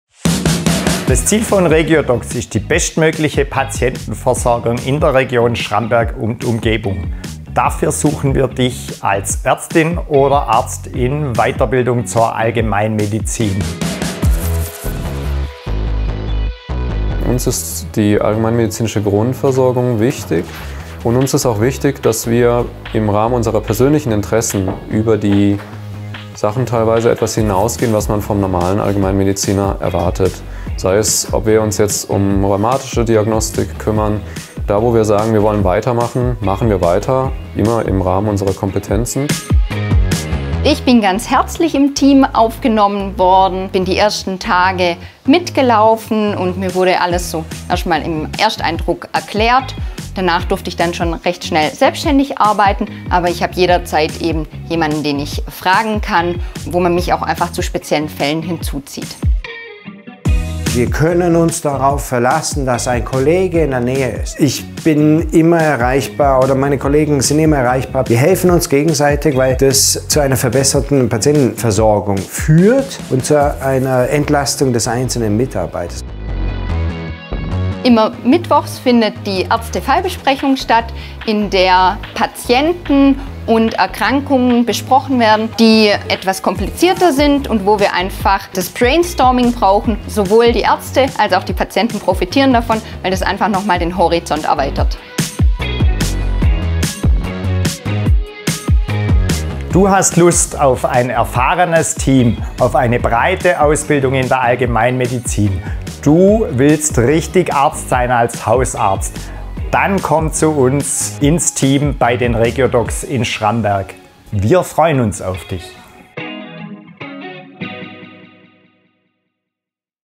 Facharzt-Weiterbildung zur Allgemeinmedizin | Regiodocs Schramberg Recruiting
Komm ins Team der Regiodocs in Schramberg und Umgebung. Wir suchen Dich für unser kompetentes und freundliches Team als Facharzt zur Weiterbildung zum Allgemeinmediziner (m/w/d). Als Hausärzte sehen wir es als unsere Aufgabe, die medizinischen Probleme der Patienten zu erkennen und Lösungen zu finden.

Wir sind vor Ort und ermöglichen Ihnen in unserer ländlichen Umgebung eine medizinische Versorgung in und um Schramberg herum: Wir führen mehrere vollständig ausgestattete Hausarztpraxen, die Sie schnell erreichen können. Wir sind gemeinsam für Sie da. Durch ein großes Ärzteteam werden Sie immer einen kompetenten ärztlichen Ansprechpartner in Ihrer Praxis vorfinden, auch wenn einer mal ausfällt. Schwierige medizinische Probleme diskutieren wir kollegial untereinander und finden gemeinsam Lösungen.